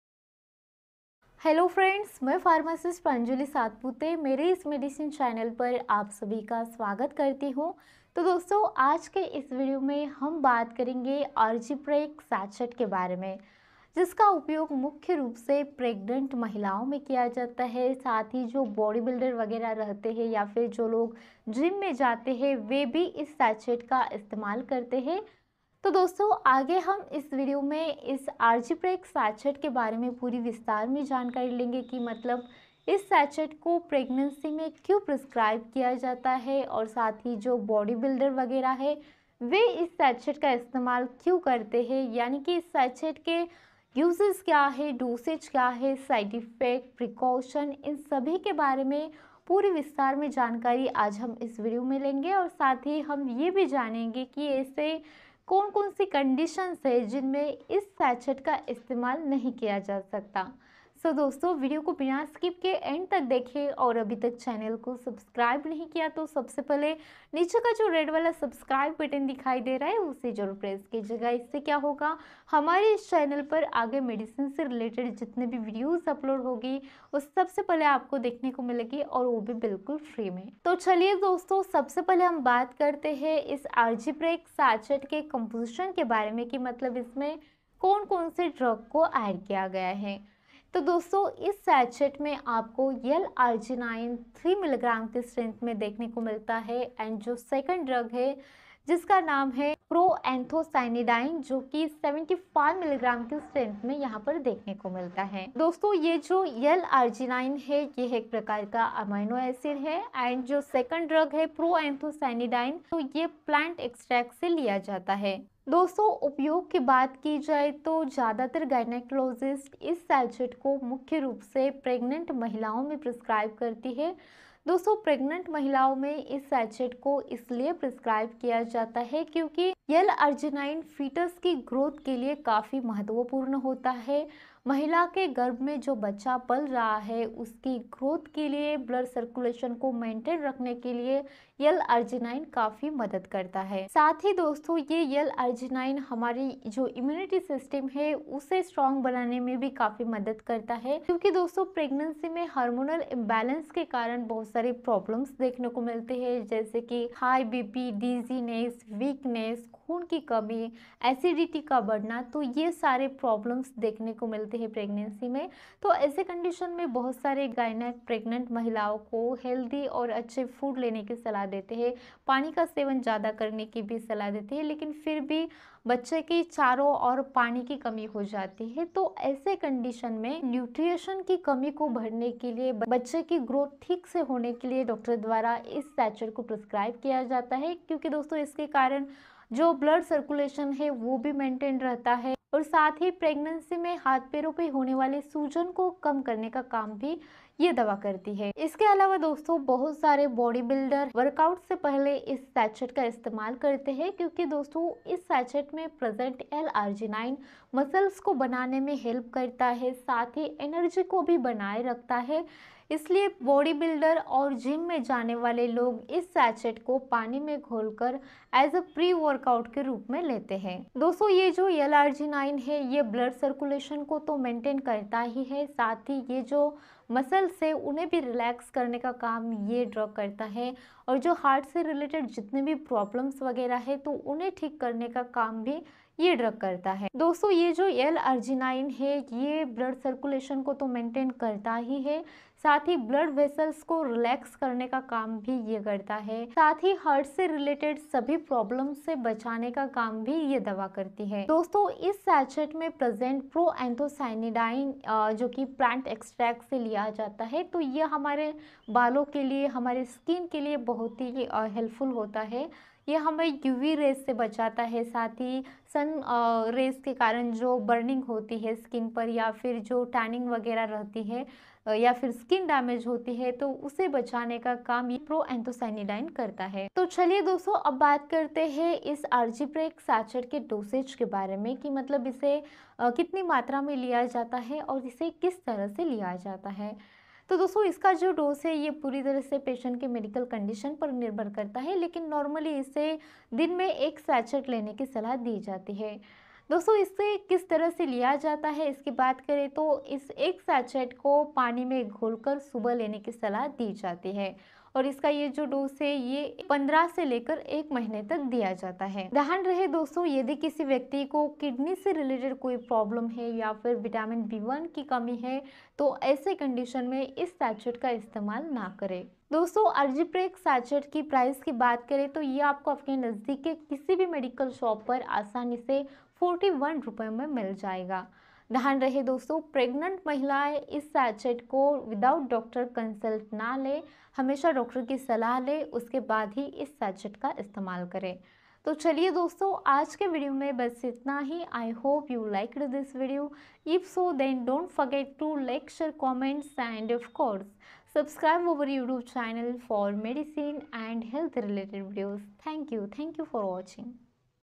Argipreg Sachet uses in pregnancy in hindi | L-arginine Proanthocynidine granules hindi | Pranjali
Hello friends welcome to back our YouTube channel Today We Are Discussing About Argipreg Sachet Uses, Dosage, Side-effects and Precautions In Hindi

• Note :- Always Consult Your Doctor Before Taking Any Medicine